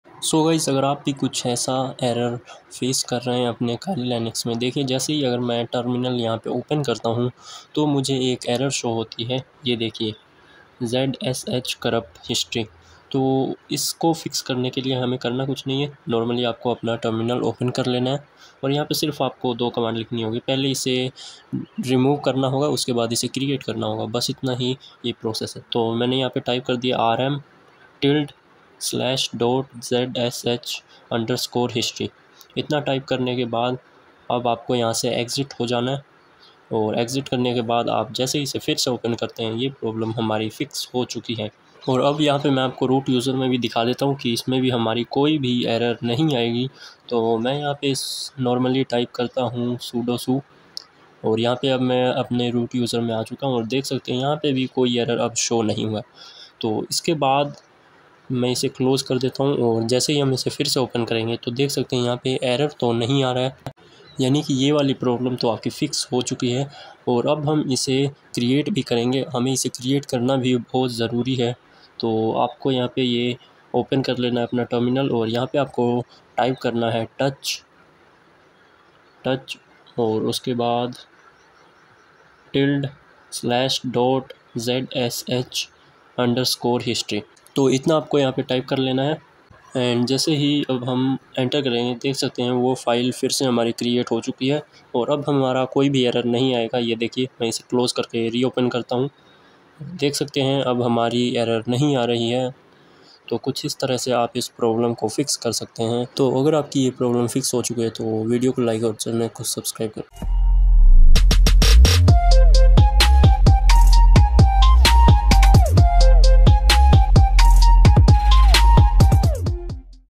zsh: currupt history file problem solution || Troubleshooting Zsh: Fixing Corrupt History File Issue
Are you encountering the frustrating "zsh: corrupt history file" error whenever you open your terminal? Don't worry, you're not alone! In this video, we delve into the common issue of corrupt history files in the Z shell (Zsh) and provide step-by-step guidance on how to resolve it. From understanding the root cause to implementing effective solutions, we've got you covered. Say goodbye to those pesky error messages and regain control of your terminal with our comprehensive troubleshooting guide. Watch now and streamline your command-line experience with ease!